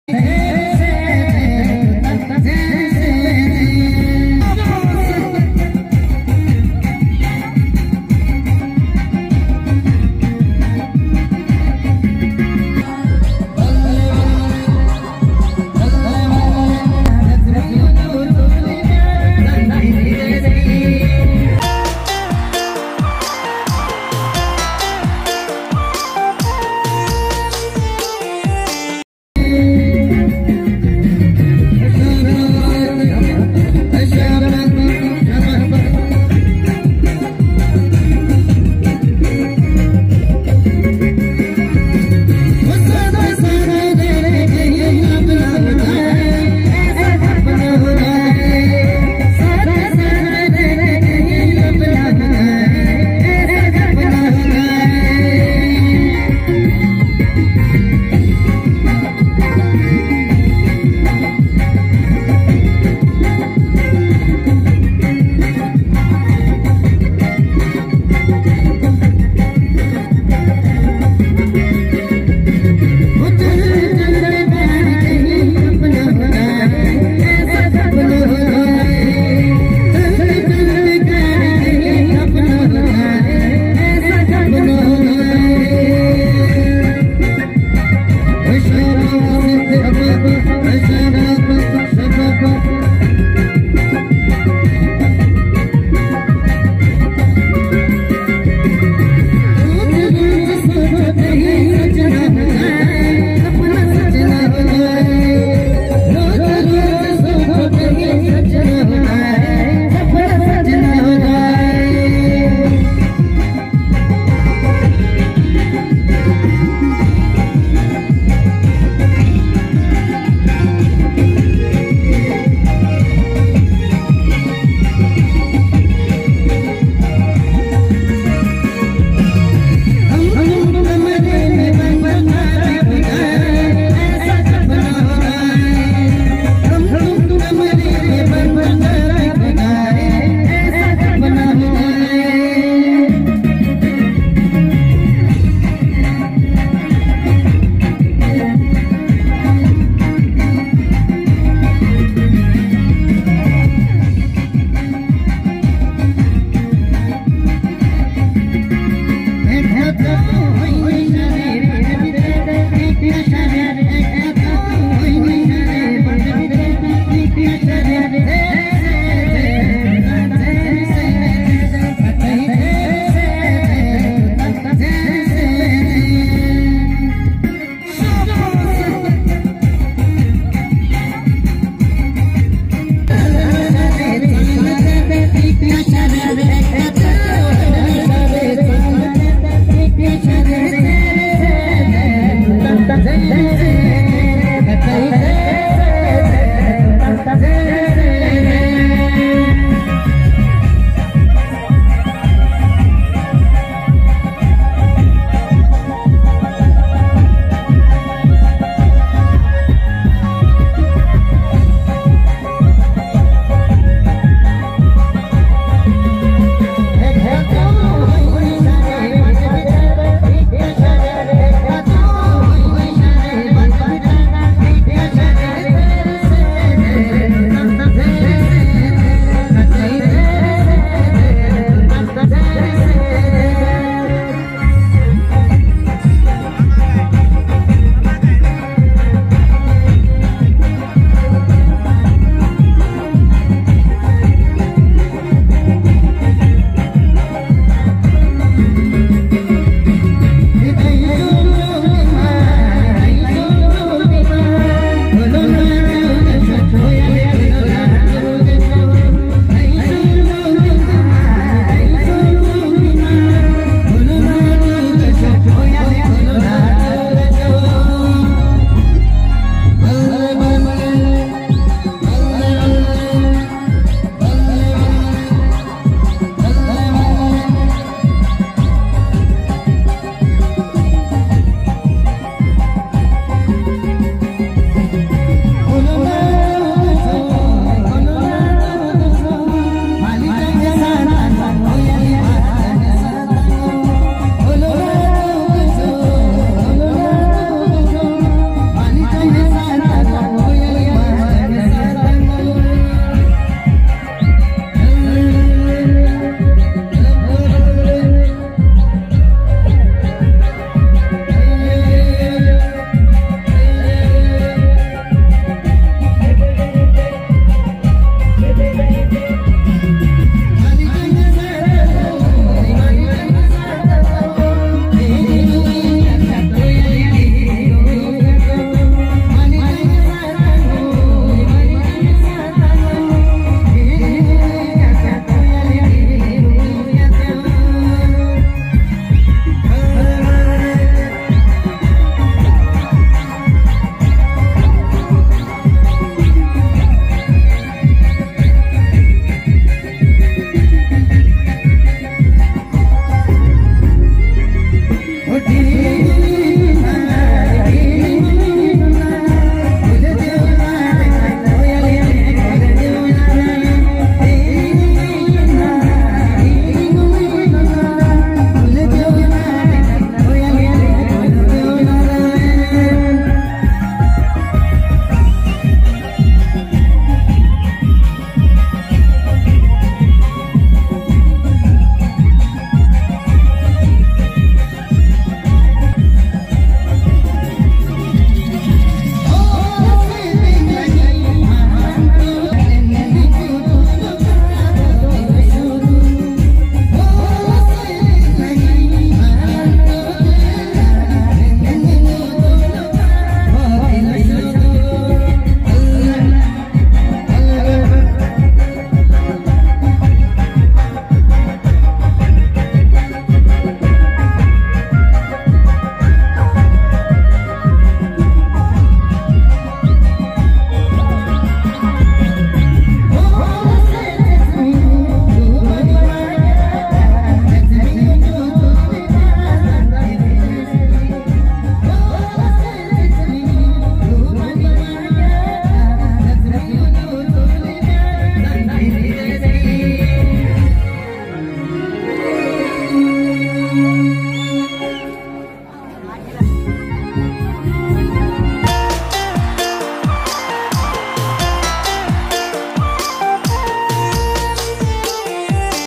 New Kinnauri Wedding Dance || Kaviraj weds Jugnu || Bhaba Valley Kinnaur Himachal
New Kinnauri Wedding Dance || Kaviraj weds Jugnu || Bhaba Valley Kinnaur Himachal || Kinnauri Culture

hi frnz hope you like this video